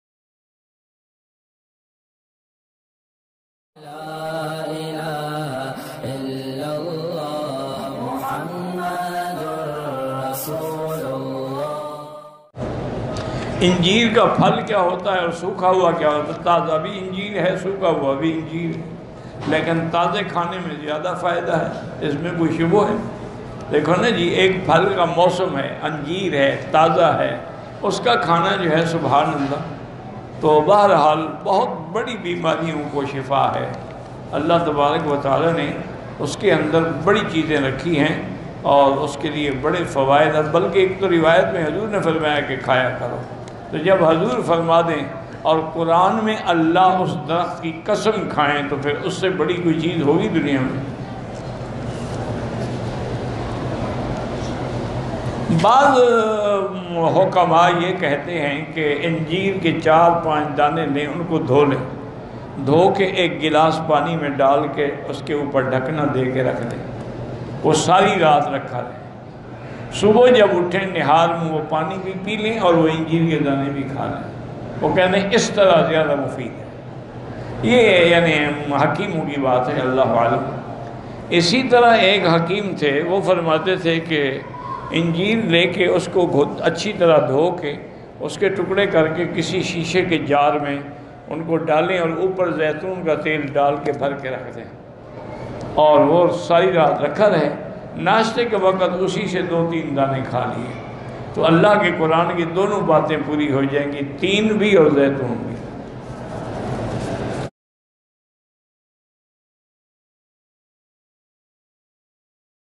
Health Benefits Of Anjeer | Anjeer Ke Faeday | Maulana Makki Al Hijazi | Islamic Group
(Ahem Sawal Jawab)

1. Anjeer Mein Badi Bimariyon Ka Ilaaj

2. Anjeer Ko Khane Ka Sahih Tareeqa

3. Anjeer Ke Faeday

BY Sheikh Makki Al Hijazi Sahab